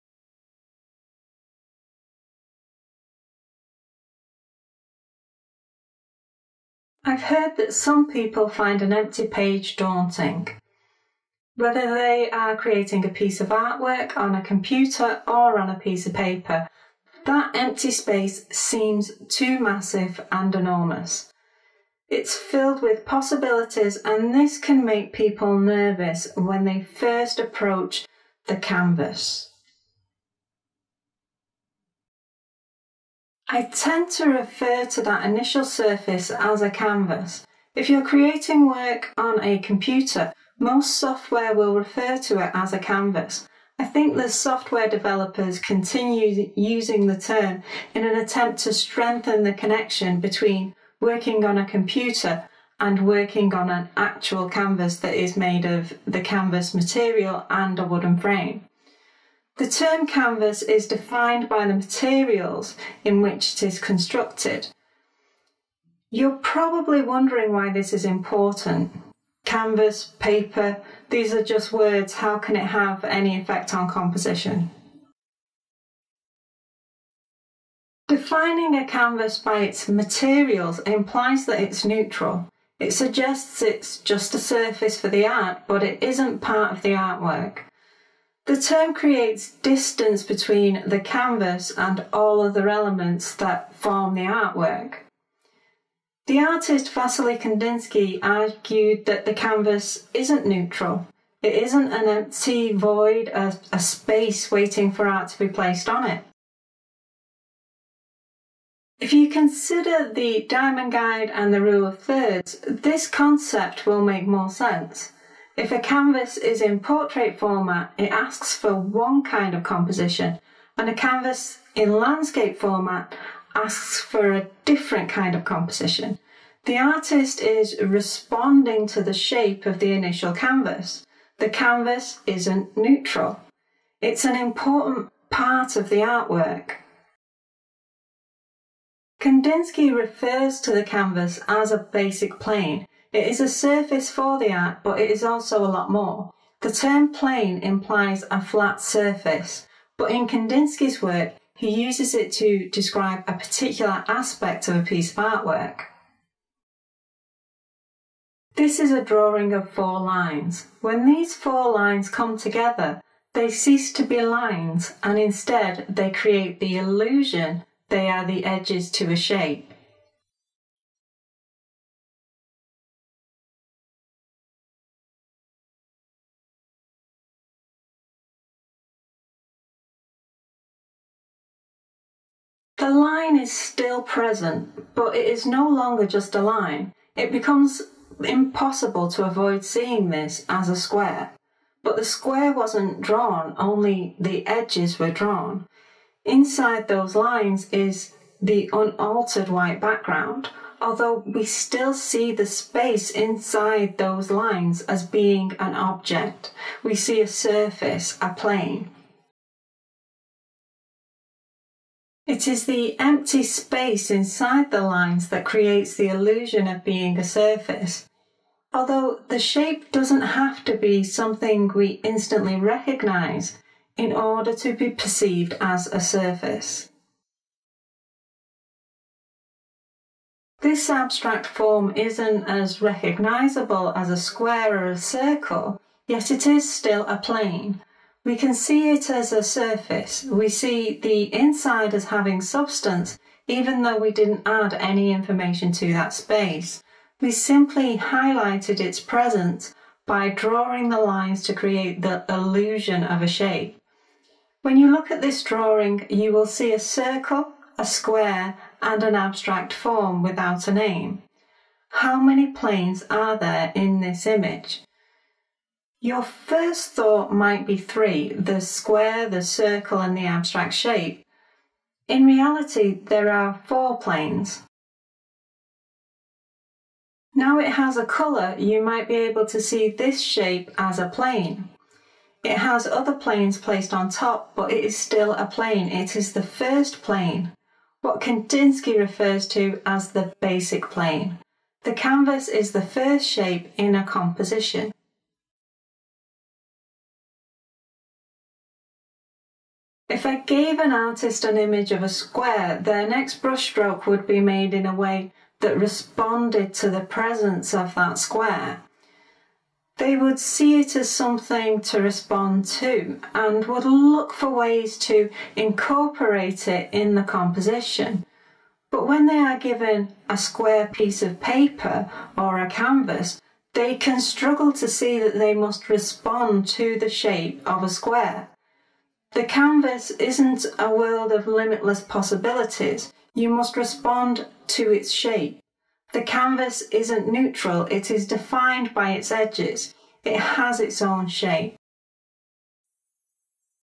C2.09 Kandinsky’s Theory
This is the 9th lecture from the C2 Advanced Art Composition course. In this video we explore Kandinsky's idea that the canvas isn't neutral.

suggested reading
Kandinsky, W. (1979). Point and line to plane. New York: Dover.